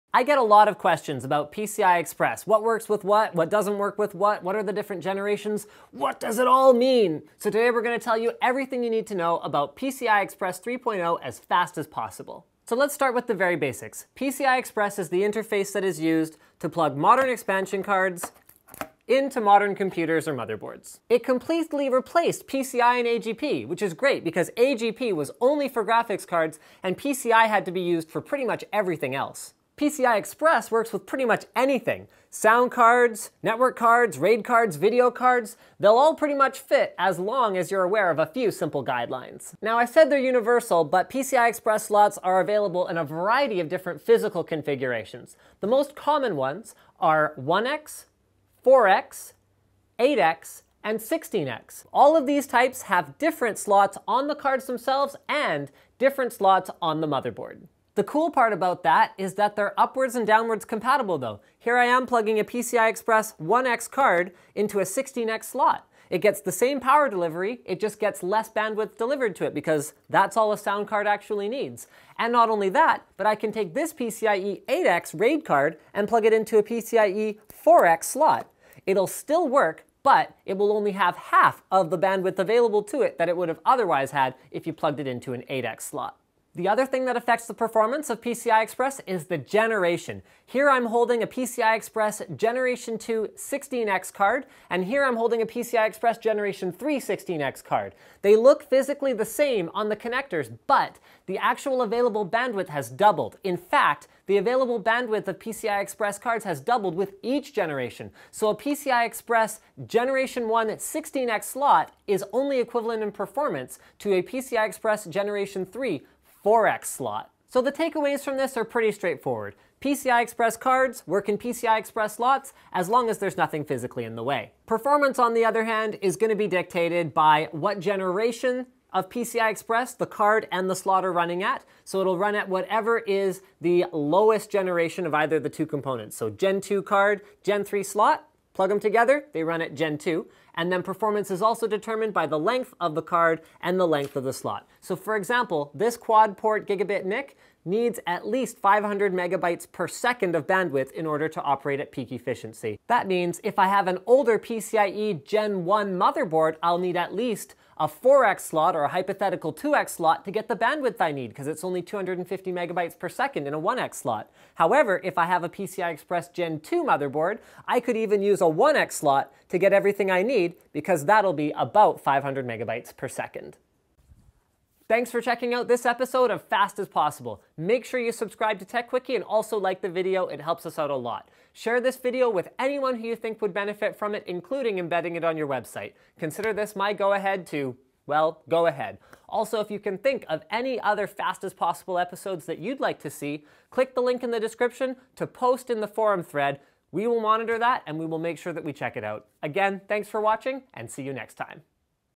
PCI Express (PCIe) 3.0 - Everything you Need to Know As Fast As Possible
PCIe compatibility and performance generates a LOT of confusion. In about 2 minutes we'll tell you everything you need to know!.